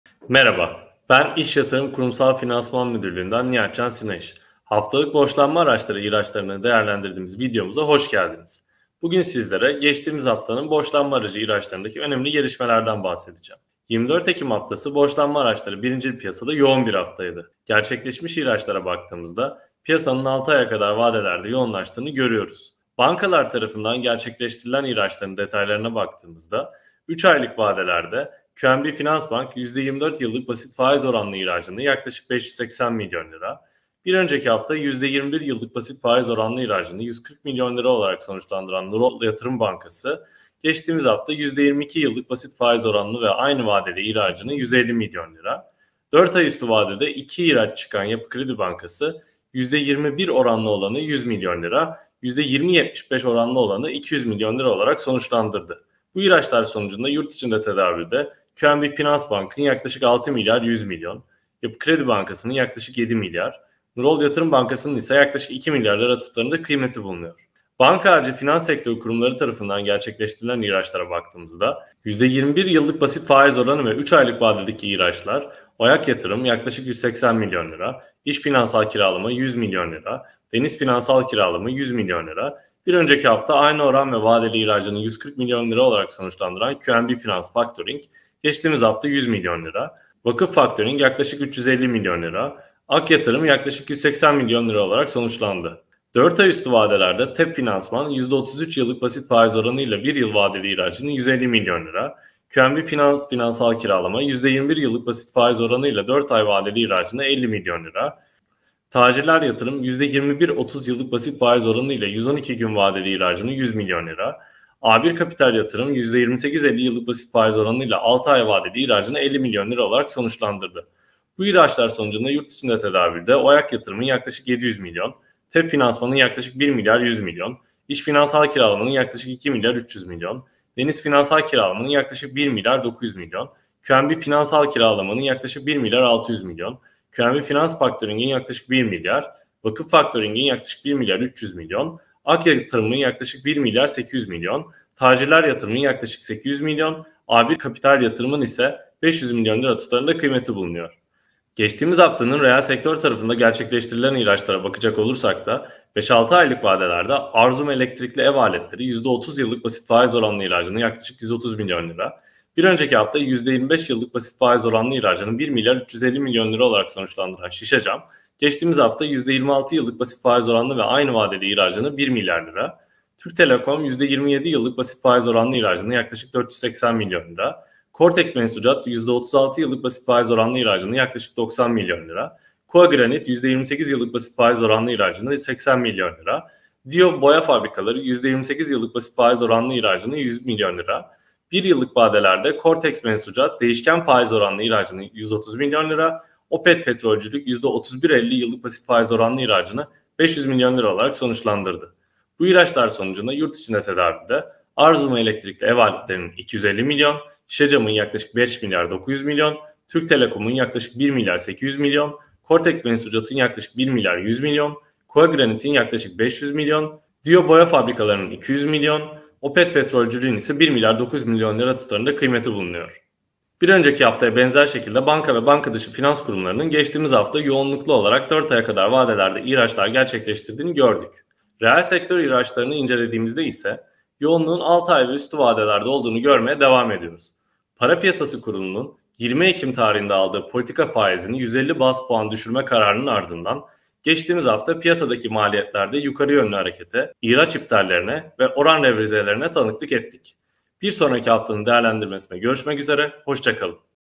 HAFTALIK BORÇLANMA ARACI İHRAÇLARI - 31.10.2022 - Nihat Can Sinayiş
📌 İş Yatırım Kurumsal Finansman Müdürlüğünden Nihat Can Sinayiş, geride bıraktığımız haftanın borçlanma aracı ihraçları hakkında bilgiler paylaşıyor.